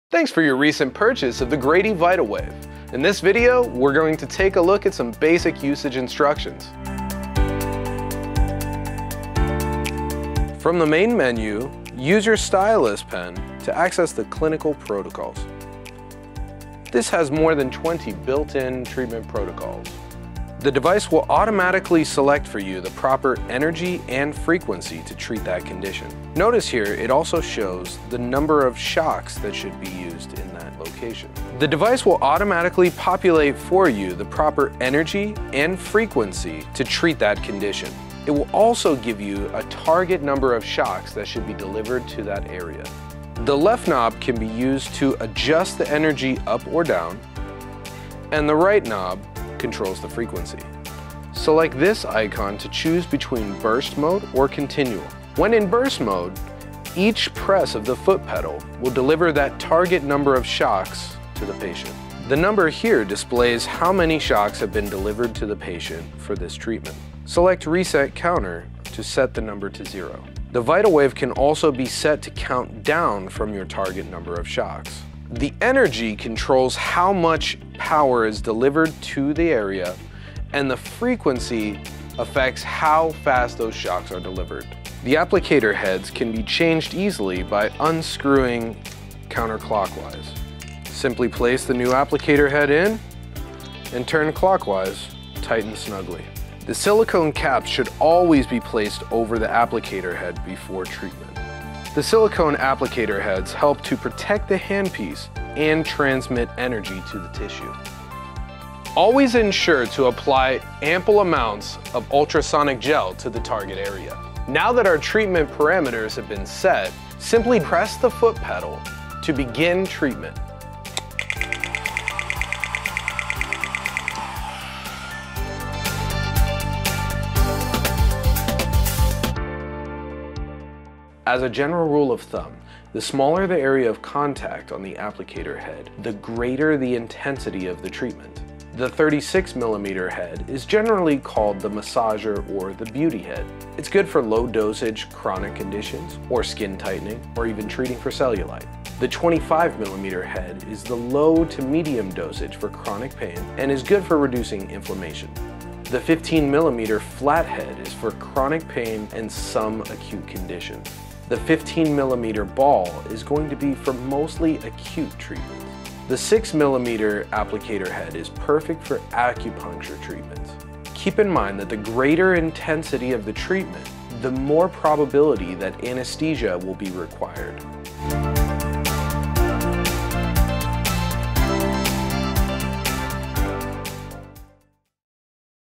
Grady Product Overview - VitalWave
The Grady Medical VitalWave is state-of-the-art technology. Through the application of acoustic waves, it provides effective rehabilitative and healing therapy. Shockwave Therapy is one of the most impactful ways to relieve pain associated with the musculoskeletal system for animals, including dogs, cats and horses.

The device consists of three parts: the main unit, the applicator and foot-switch.
Main Unit – The VitalWave system contains the main micro-computer and software for controlling the entire system. It also includes a user encyclopedia and the operation guide.
Applicator – this ergonomically-designed applicator is easy to handle and features a sturdy design that will handle years of use.
Footswitch – Sturdy foot pedal triggers the system to work.